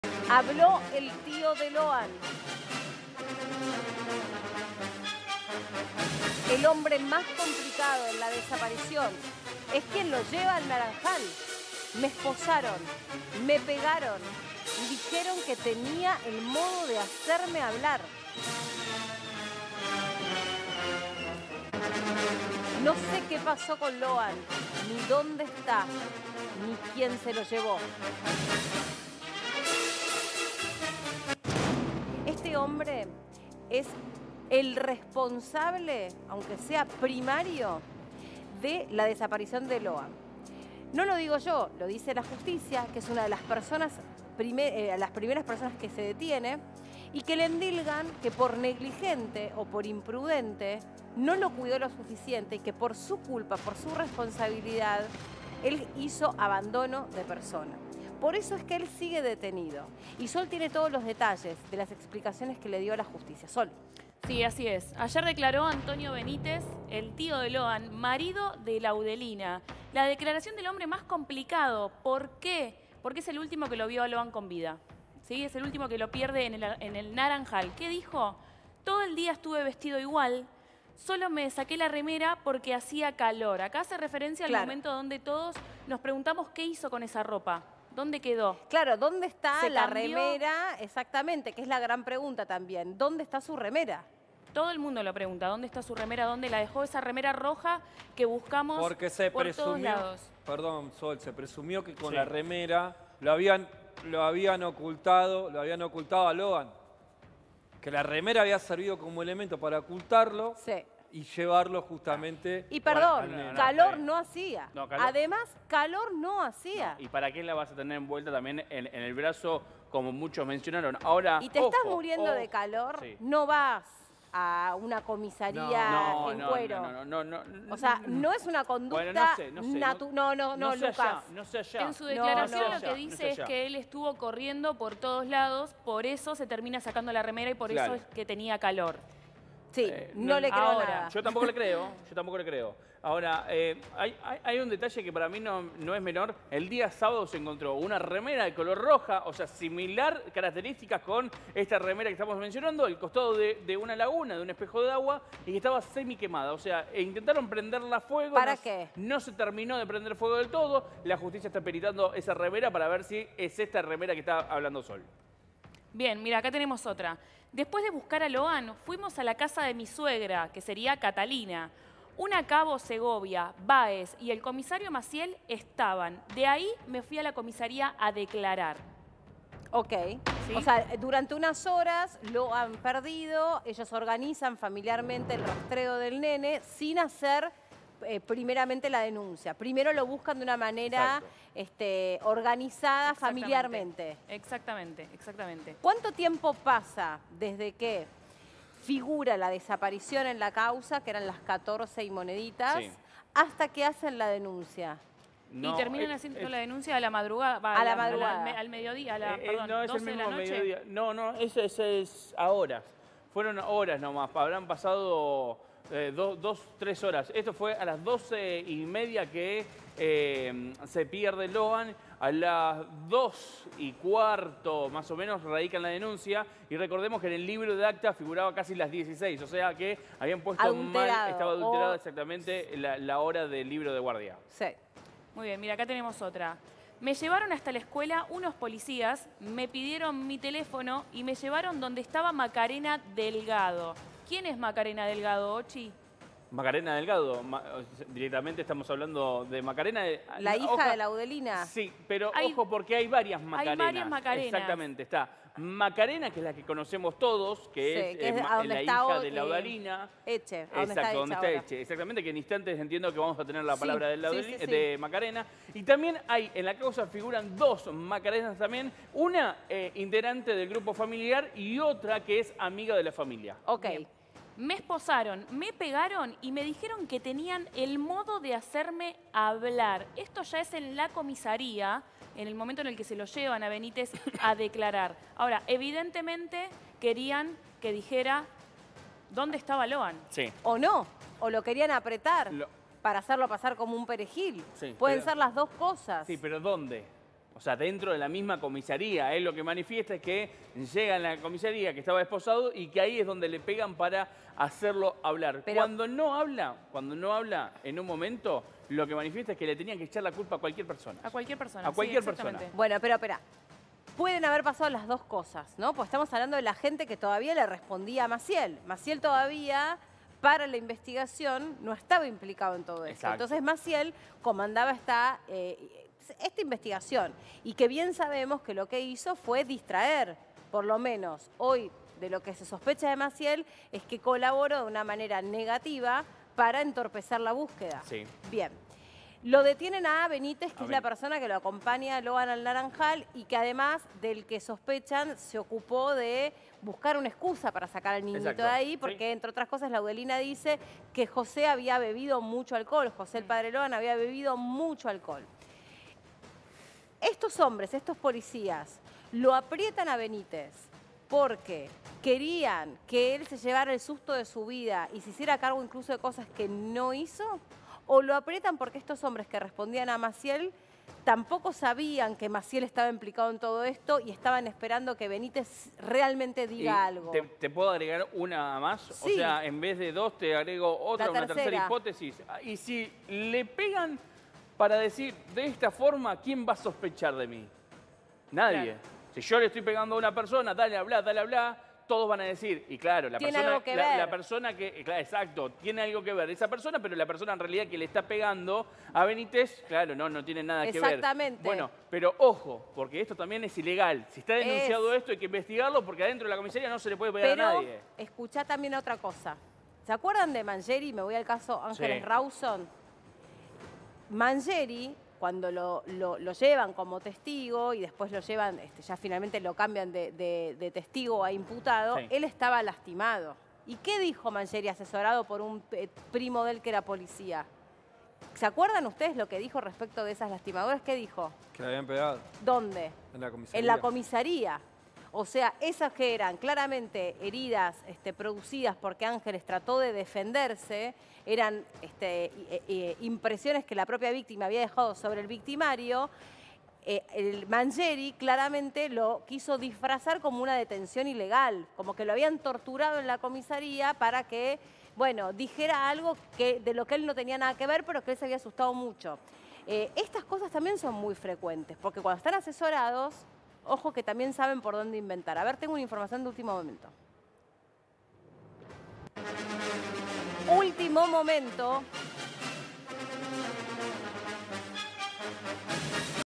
Tío de Loan: "En la comisaría me esposaron y me pegaron"
El familiar de Loan Peña declaró y aclaró que se sacó la remera por el calor que hacía y porque estuvo corriendo todo el día para encontrar al pequeño, desaparecido hace 35 días.